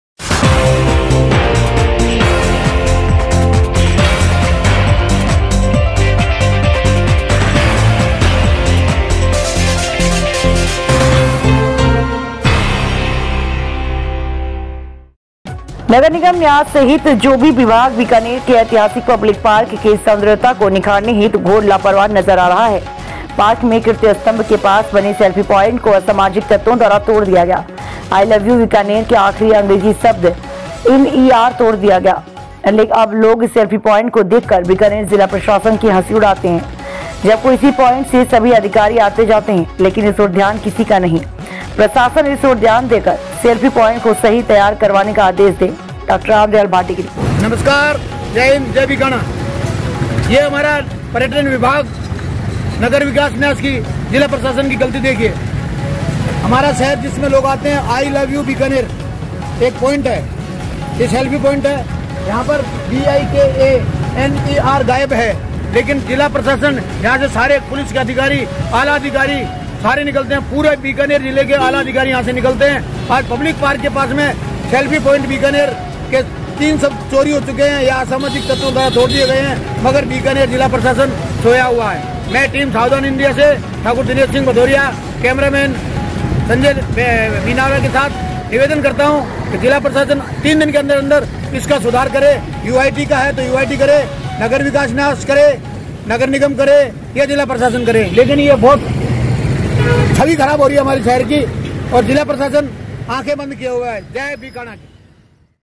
बीकानेर। नगर निगम न्यास सहित जो भी विभाग बीकानेर के ऐतिहासिक पब्लिक पार्क की सौंद्रीयत्ता को
बीकानेर। नगर निगम न्यास सहित जो भी विभाग बीकानेर के ऐतिहासिक पब्लिक पार्क की सौंद्रीयत्ता को निखारने हेतु घोर लापरवाह नजर आ रहा है। पार्क में कीर्ति स्तंभ के पास बने सेल्फी पॉइंट को असामाजिक तत्वों द्वारा तोड़ दिया गया है।आई लव यू बीकानेर के आखरी अंग्रेजी शब्द एन इ आर तोड़ दिया गया है। अब लोग सेल्फी पॉइंट को देख कर बीकानेर जिला प्रशासन की हंसी उड़ाते हैं। जब को इसी पॉइंट से सभी अधिकारी आते जाते हैं। लेकिन इस और ध्यान किसी का नही गया। प्रशासन इसऔर ध्यान देकर सेल्फी पॉइंट को सही तैयार करवाने के आदेश दे। डॉक्टर राम दयाल भाटी की रिपोर्ट